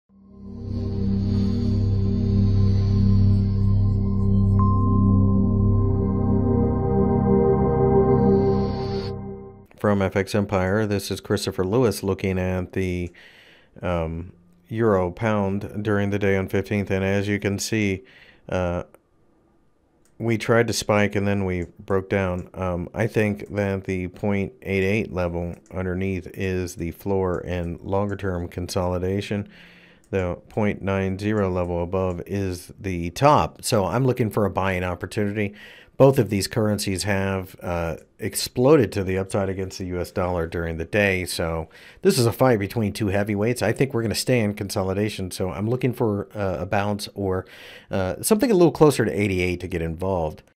EUR/GBP Technical Analysis for January 15, 2018 by FXEmpire.com
The EUR/GBP pair has tried to rally during the trading session on Friday, but rolled around rather drastically. For the full article go - EUR/GBP Forecast January 15, 2018, Technical Analysis.Find more information about Forex and Commodities News Technical Analysis Fundamental Analysis and Brokers on FX Empire